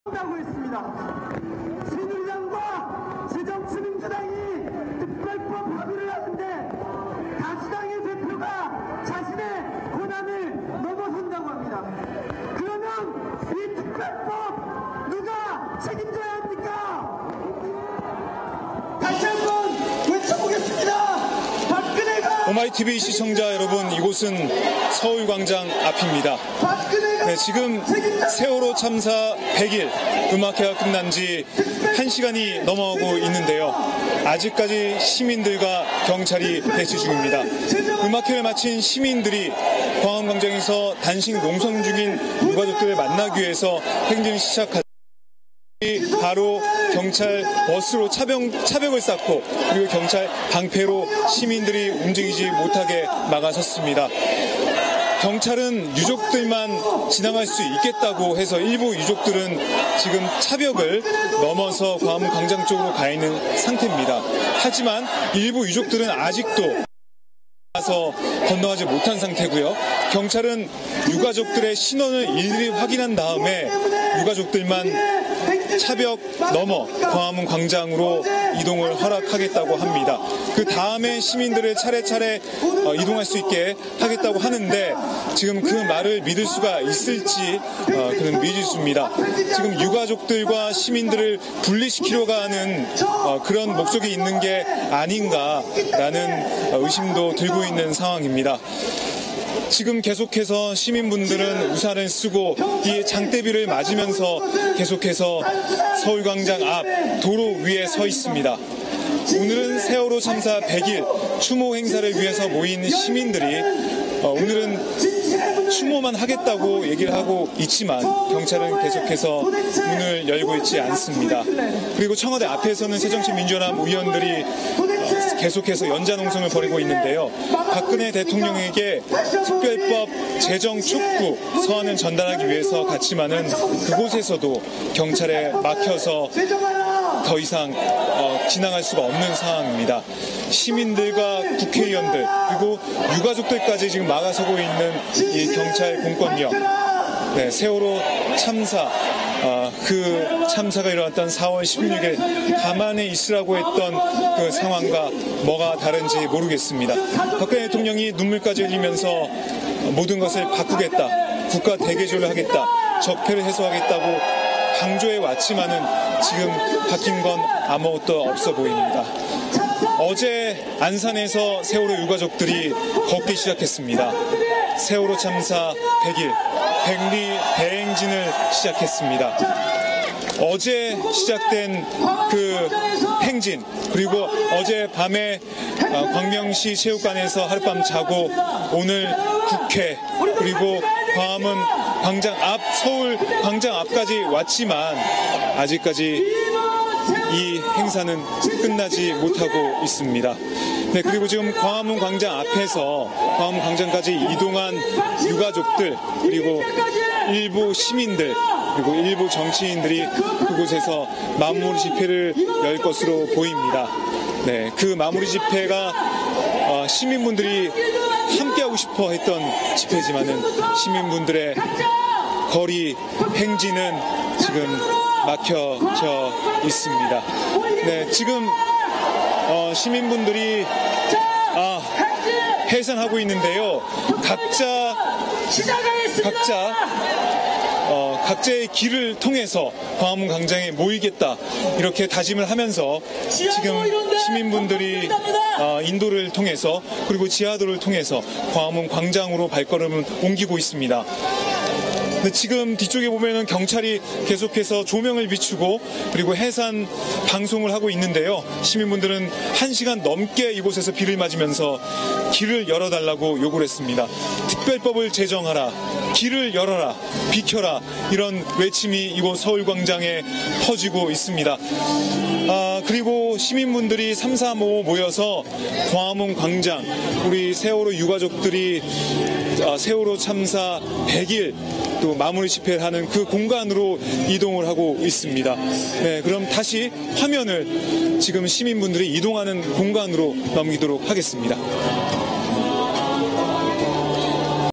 [세월호 행진 18] 경찰, 유가족·시민 행렬 차벽으로 막아
24일 밤 세월호 특별법 제정을 촉구하며 1박 2일 행진을 한 유가족들과 시민들이 서울광장에서 추모 문화제에 참석한 후 유가족들이 단식 농성 중인 광화문 광장까지 행진을 이어가려고 했으나 경찰들의 차벽에 가로 막혔다.

유가족·시민들은 경찰과 한 시간 여를 대치하다 25일 새벽 광화문 농성장 쪽으로 이동했지만 경찰은 “미신고불법집회와 일반교통방해에 해당하니 즉시 해산하라”고 해산명령을 내렸다. 이에 시민들은 함성을 지르며 항의하며 몸싸움을 벌이기도 했다.

세월호 침몰 사고 유가족들과 시민들은 전날인 23일 경기 안산 합동분향소에서 ‘세월호 특별법 제정 촉구’ 1박 2일 대행진을 시작했다. 이들은 행진 둘째 날인 이날 광명시민체육관에서 출발해 국회와 서울역을 거쳐 서울광장 합동분향소까지 행진한 뒤 세월호 참사 100일 추모 콘서트에 참석했다.

오마이TV는 유가족들의 행진 전체를 생중계했으며, 이 동영상은 이날 유가족들의 행진을 동행 취재한 박정호 오마이TV 기자의 리포팅을 담고 있다.